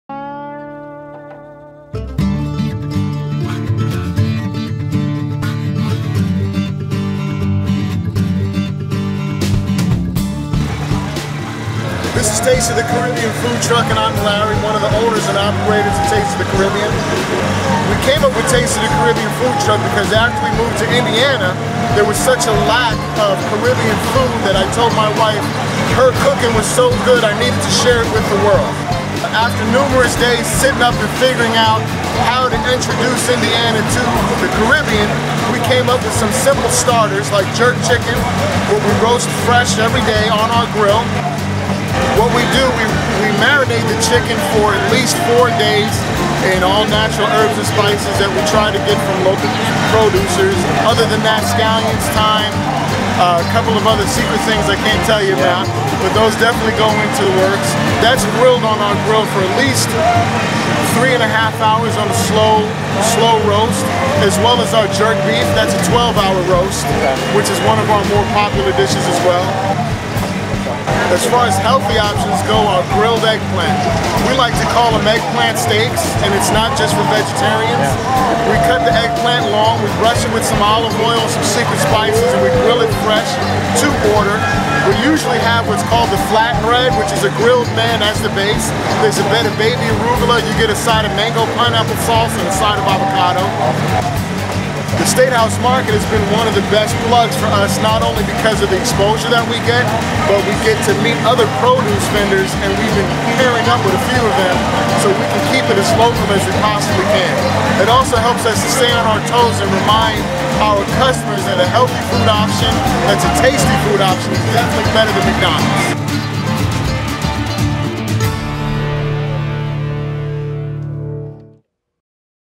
Taste of the Caribbean - Statehouse Market
Every week the Statehouse Market offers a variety of food trucks and produce vendors. This week we feature an interview with Taste of the Caribbean, the only authentic Jamaican cuisine food truck in Indianapolis. Vendors and food trucks attend the Statehouse Market and provide a fresh lunch and shopping for a healthy dinner just outside the Statehouse for Downtown Indianapolis.

The Statehouse Market will run every Thursday through October 9th.
10:30 a.m. - 1:30 p.m.
Robert Orr Plaza and Senate Ave.
Indianapolis, Indiana